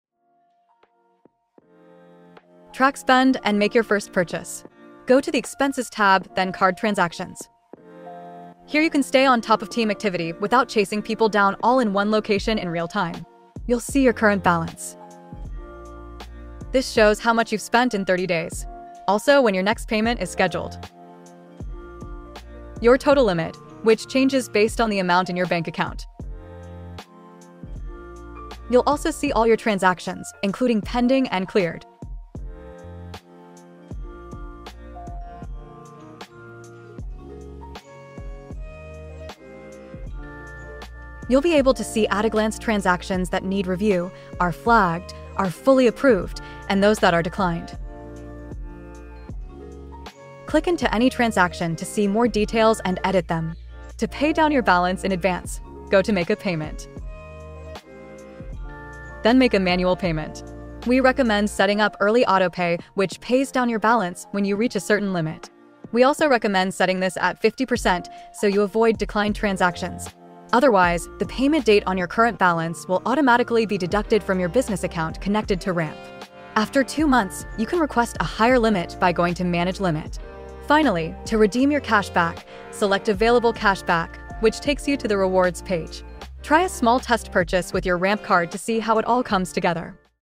Track spend in Ramp & make a payment
Explore the Ramp expenses dashboard to see how spend is tracked in real time. Learn how to review transactions, set up early autopay, and make your first test purchase to activate your card and complete setup.

Not on Ramp yet? Learn more and schedule a live demo to see how Ramp can save your business time and money